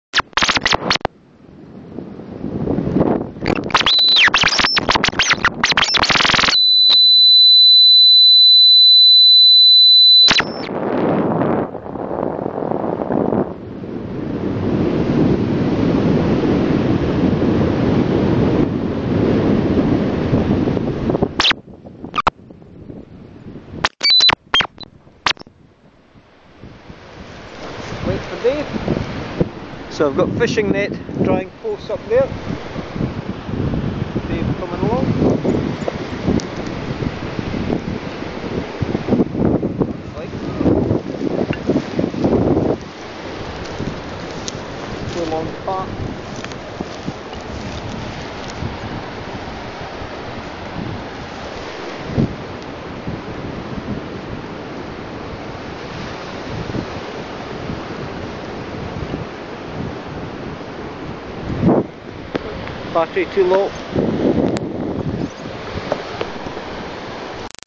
GolfRidge InFlight 2
Paragliding along the coast at Montrose